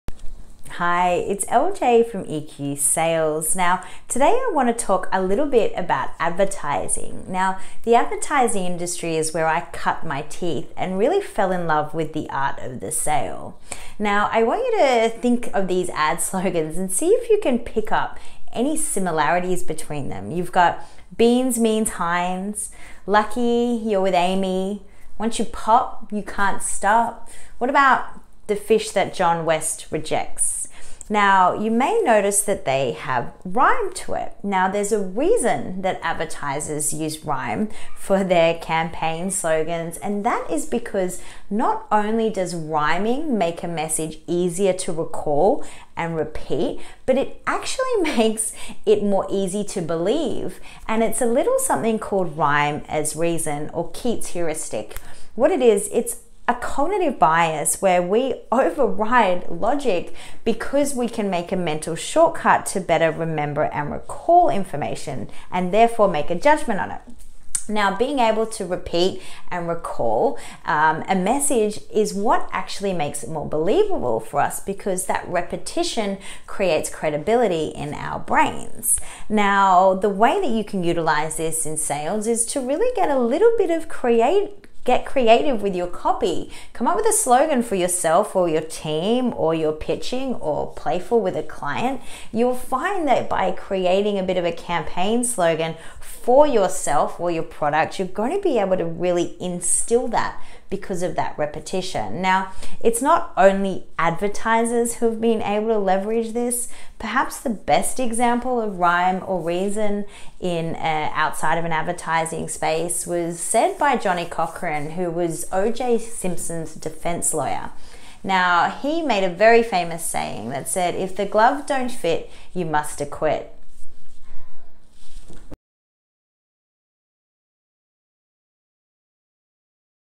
Why rhyming slang can actually help you boost sales.
Did you know that if a saying rhymes then we are more likely to believe it? It is one of the key drivers of messaging in advertising and you can leverage it to your advantage in sales.